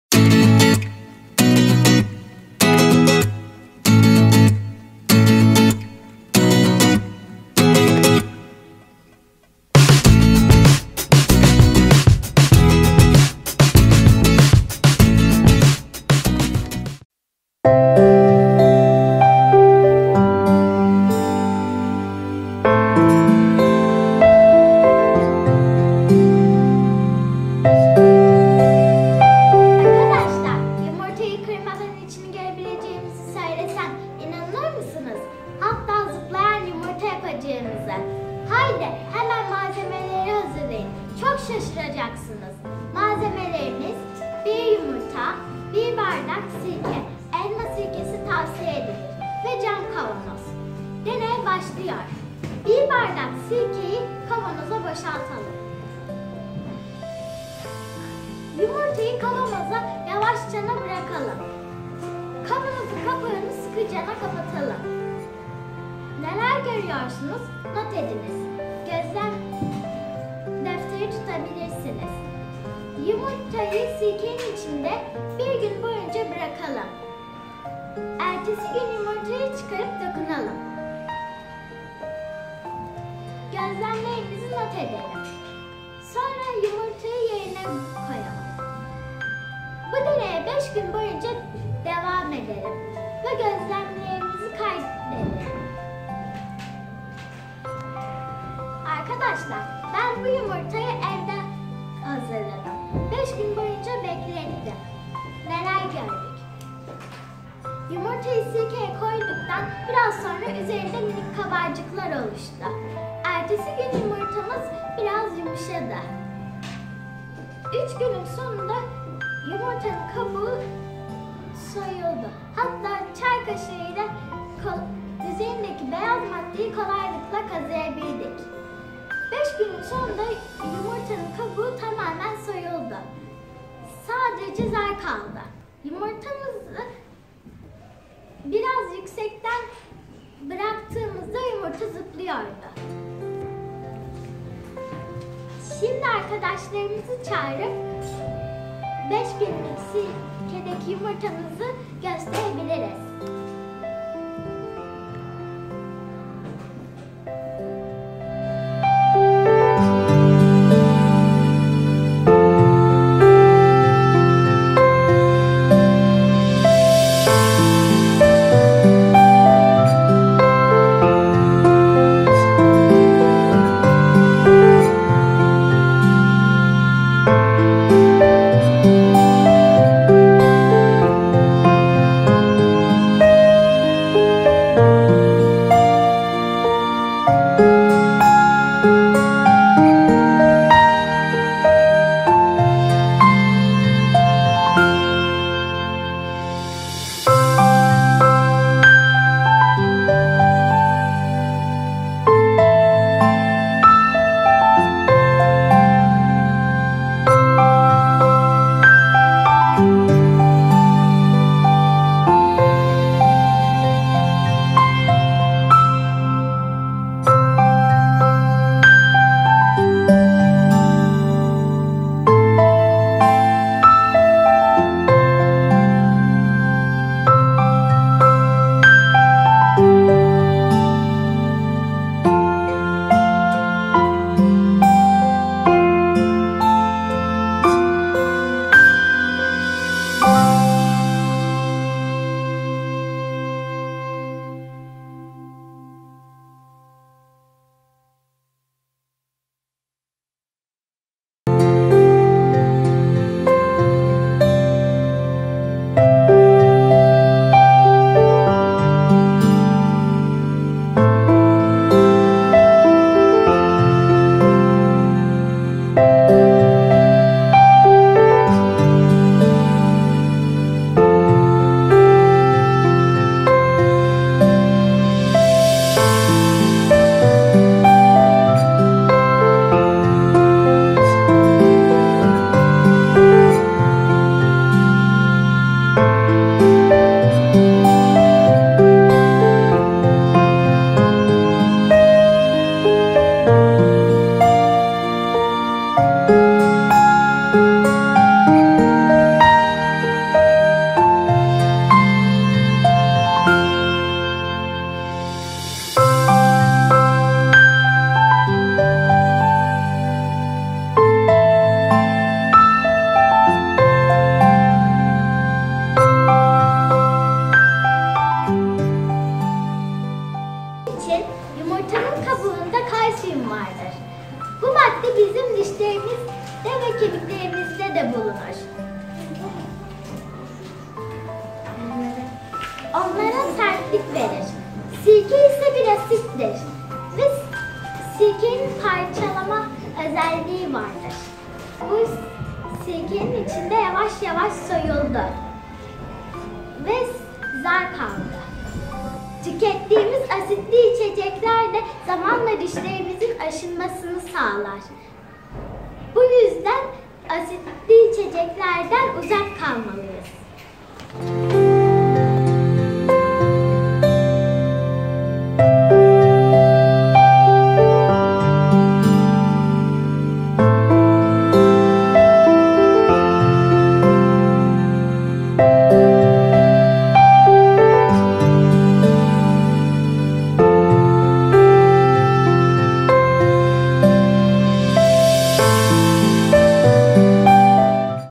LASTİK YUMURTA DENEYİ, DENEY YAPALIM PROJESİ, ÇOCUKLARLA SINIFTA YAPILAN DENEYLER
Sirkenin yumurta üzerindeki etkisini çok güzel bir biçimde ortaya koyan deney bizlere asitli gıdalardan niçin uzak durmamız gerektiğini gösteriyor

"İçeriklerimiz özgün ve bize aittir.  "